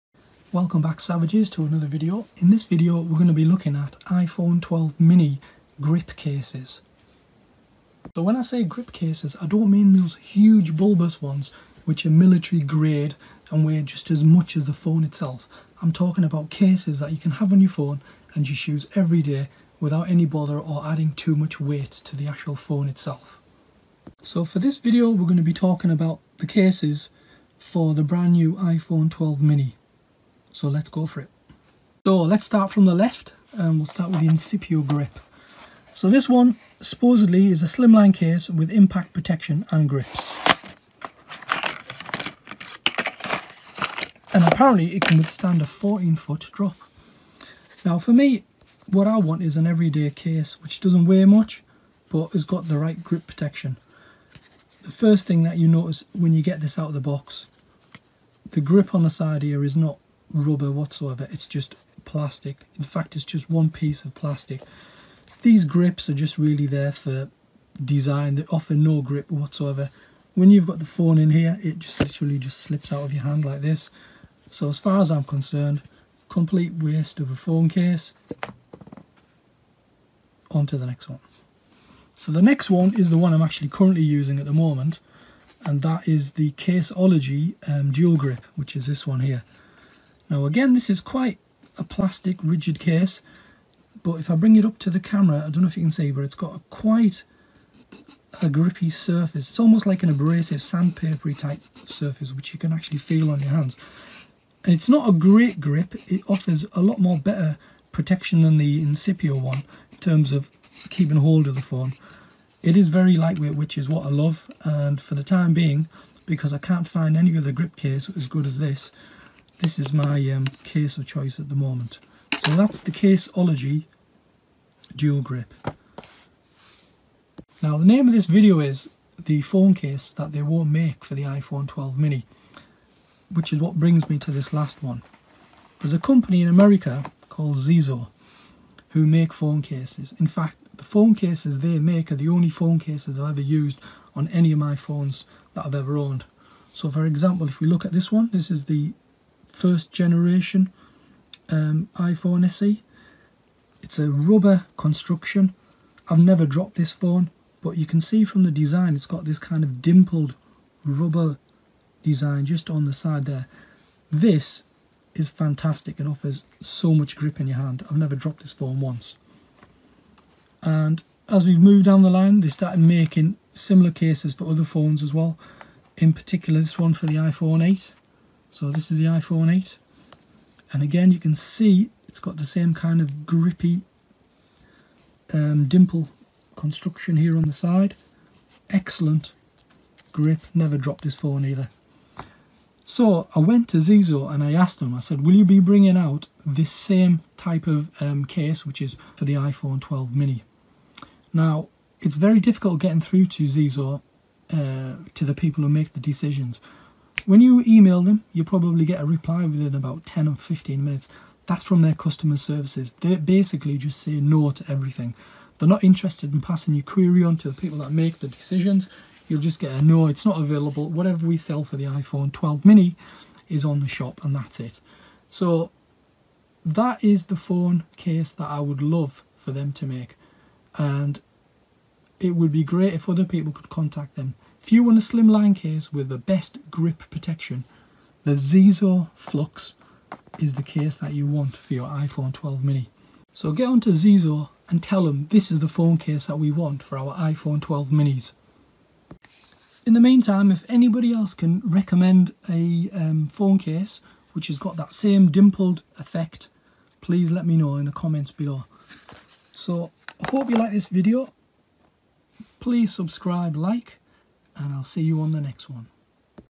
iPhone 12 Mini Grip Cases and the Best Grip Case they won't make
We review the best compact, yet grippy phone cases for the iPhone 12 mini

This isn't about military grade cases which weight more than the phone, we look at the every day cases with minimal weight, yet offer the best hand grip.

◉Incipio Grip
◉Caseology Dual Grip

if you want the Best Grip case bar none, then get onto Zizo and tell them what's up...

Are there better grip cases out there? Have you seen an iPhone 12 mini dimple-sided rubber grip, if so, then share on this channel.